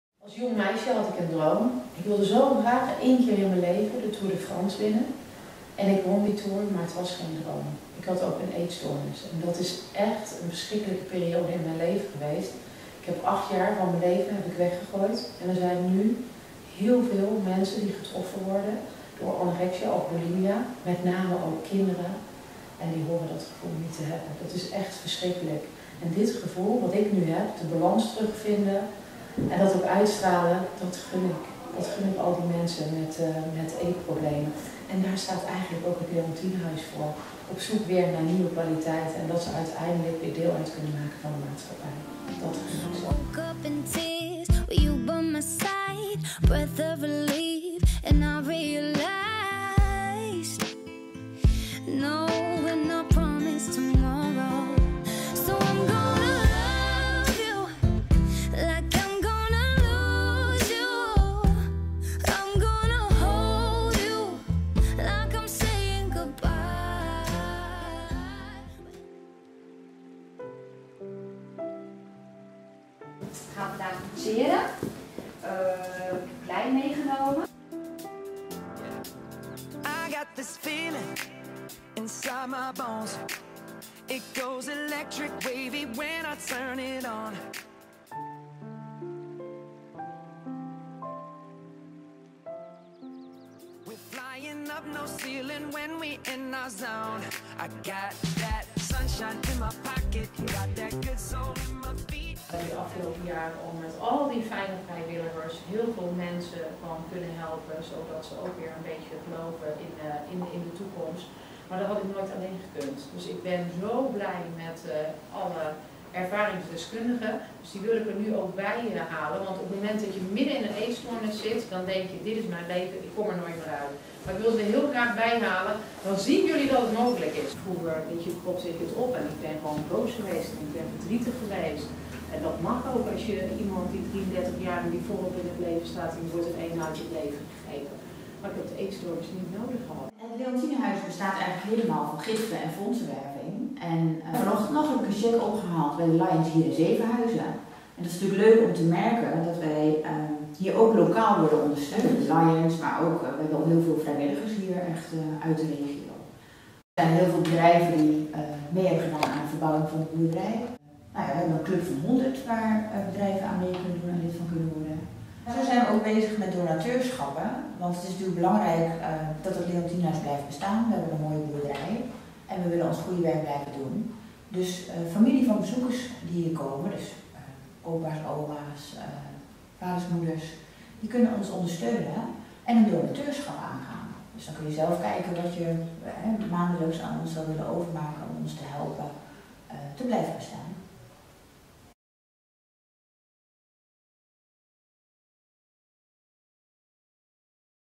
Leontienhuis sfeerimpressie
Sfeerimpressie gemaakt van het Leontienhuis in Zevenhuizen. Goed om te zien, dat er zoveel mensen geholpen kunnen worden; mooie locatie en fijne sfeer.